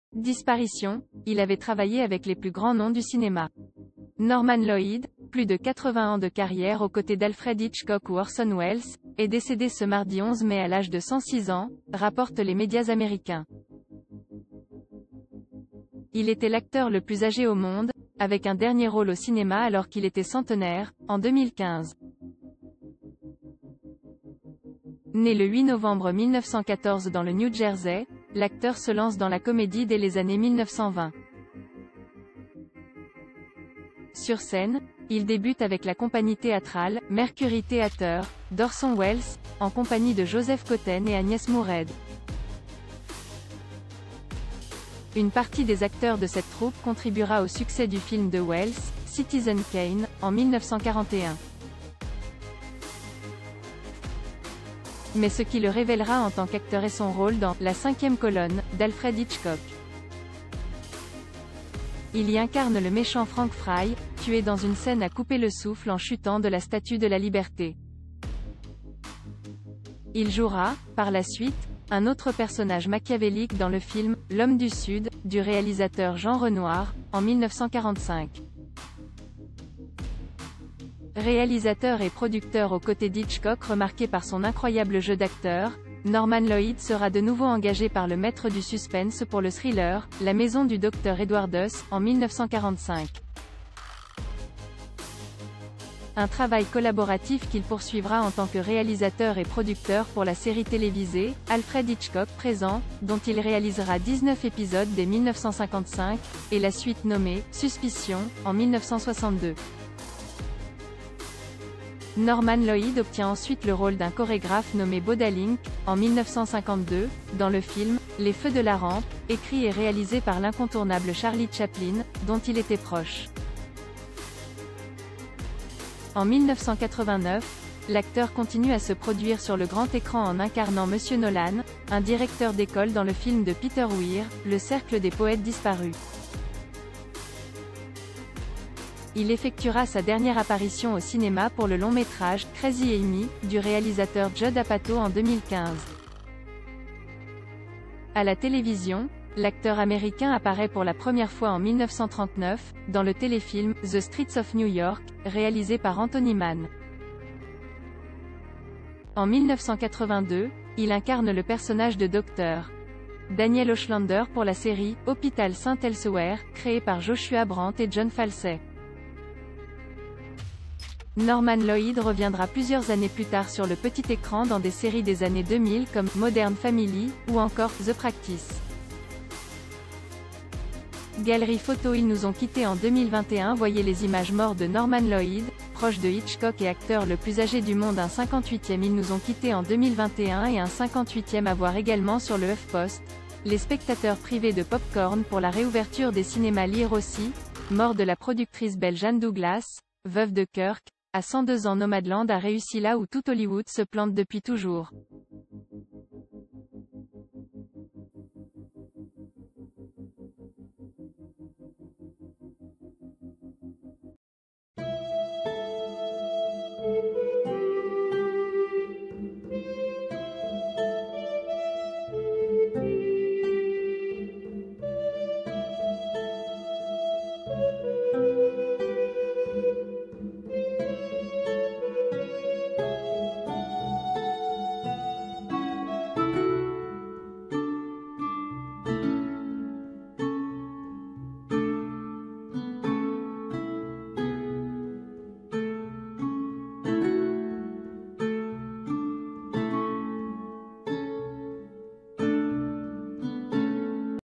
L'acteur américain était célèbre pour avoir incarné le rôle de Frank Fry dans le film "La Cinquième
DISPARITION - Il avait travaillé avec les plus grands noms du cinéma. Norman Lloyd, plus de 80 ans